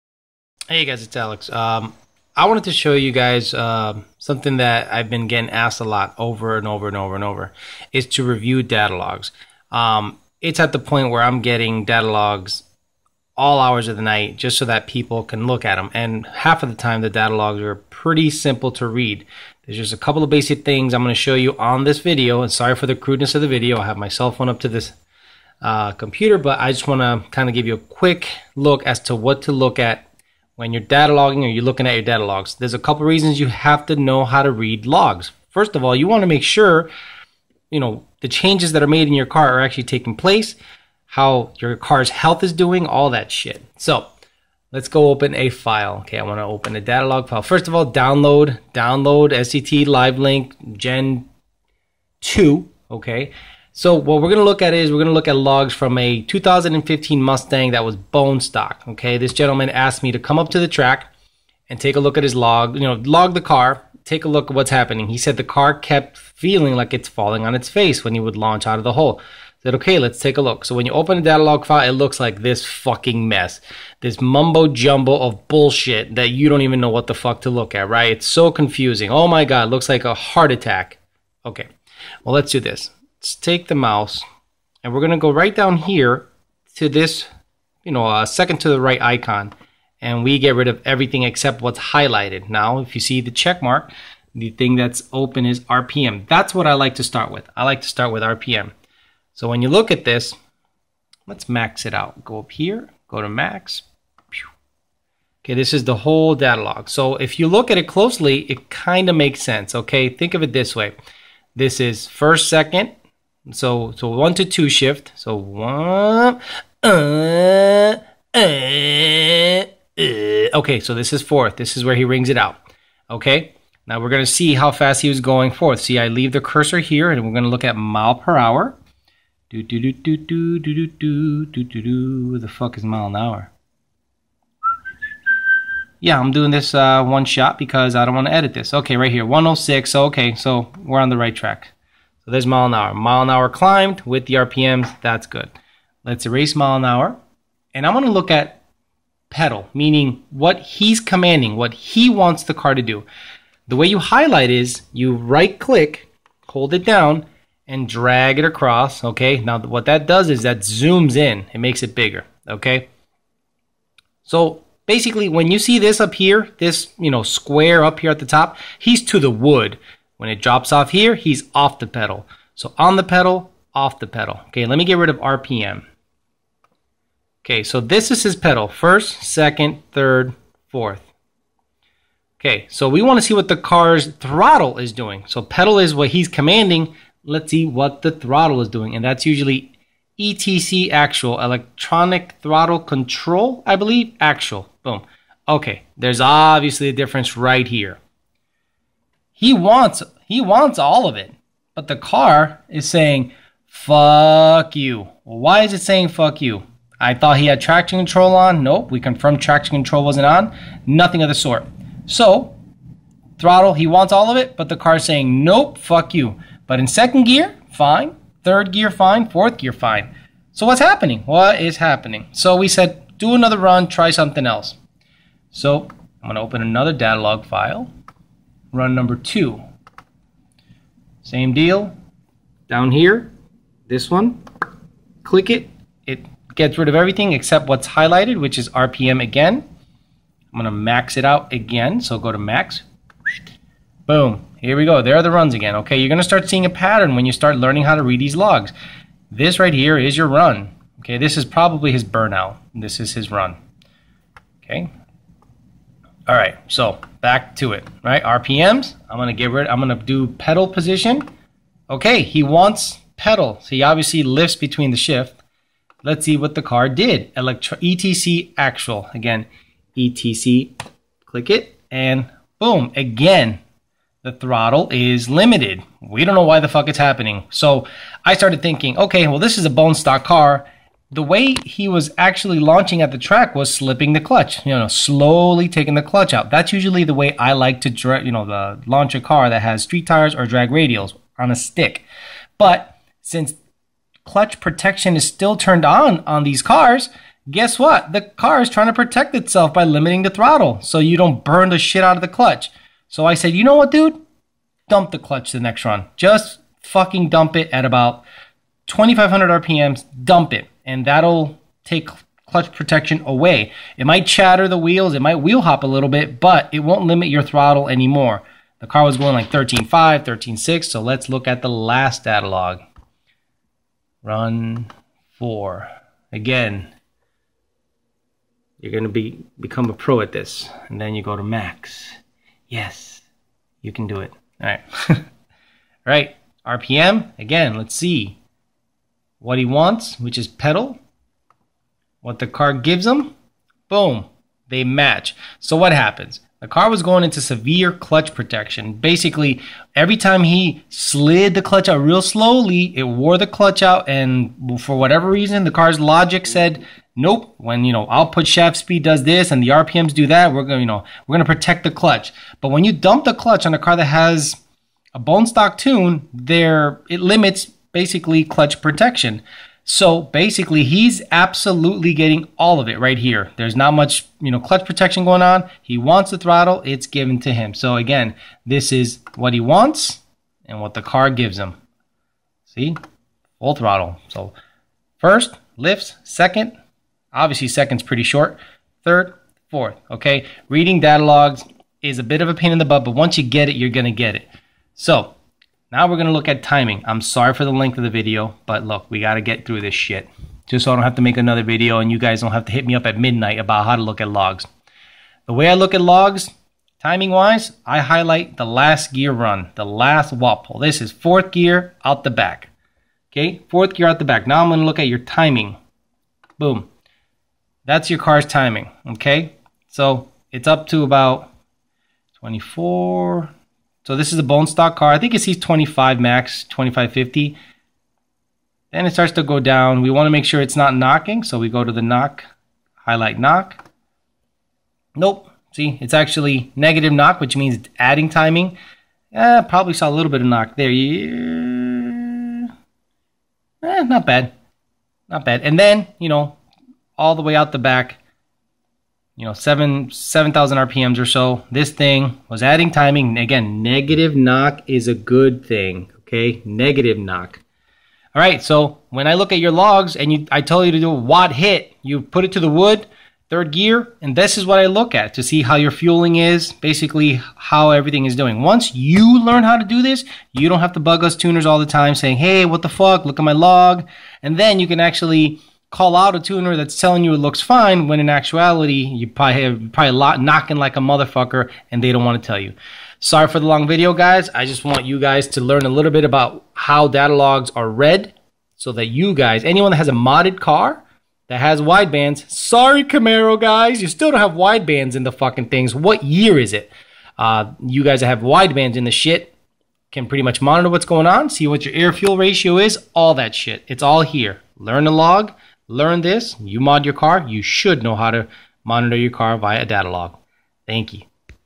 Datalogging Tutorial
Showing you a quick look into what i do most of the day. Look at logs.